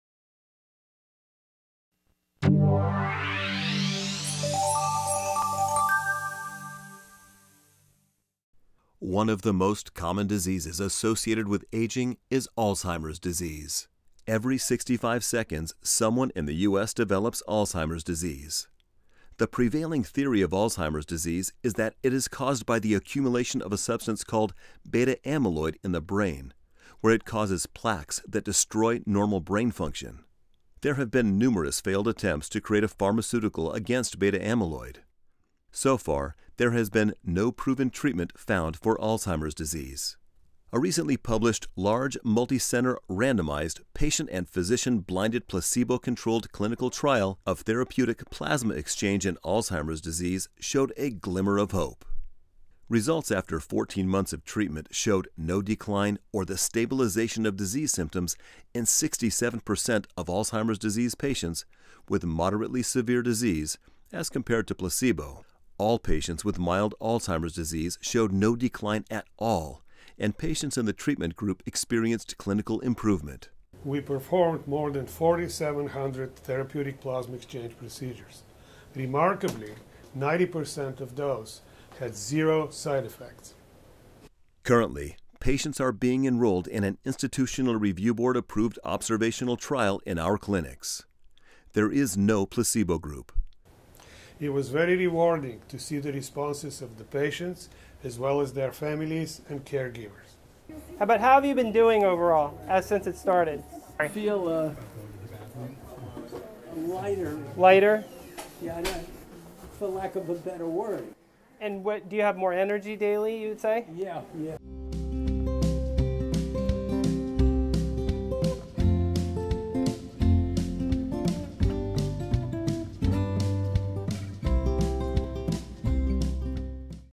Azheimer's Disease and Therapeutic Plasma Exchange (TPE)
Therapeutic Plasma Exchange (TPE) is the first treatment for Alzheimer's Disease that is scientifically proven to arrest the progression of the disease. Dr. Kiprov discusses the experience from a large, placebo controlled clinical trial.